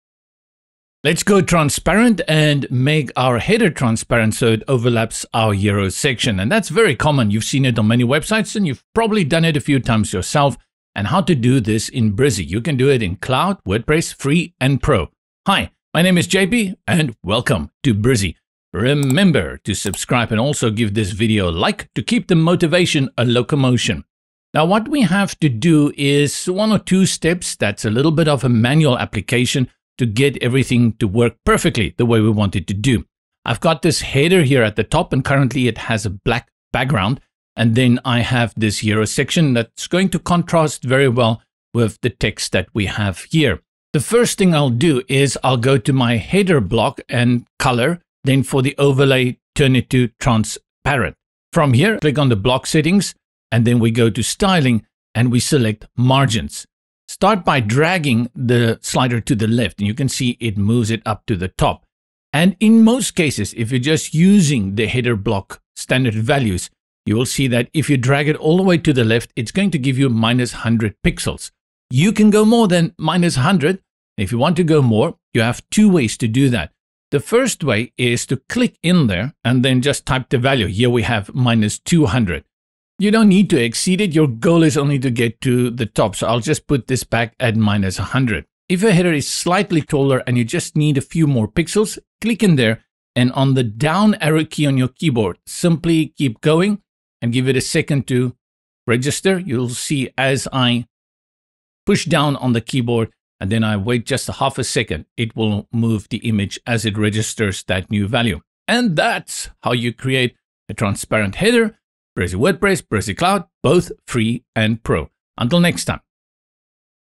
How to Create a Transparent Header in Brizy
In this video, we'll show you how to create a transparent header for your web pages in Brizy with just a few clicks.

Creating a transparent header in Brizy is a great way to make your pages look more professional and organized. It's also a great way to increase spiders' and search engines' crawling speed. We'll walk you through the steps needed to create a transparent header in Brizy, and you'll be able to see the effects it has on your website in just a few minutes!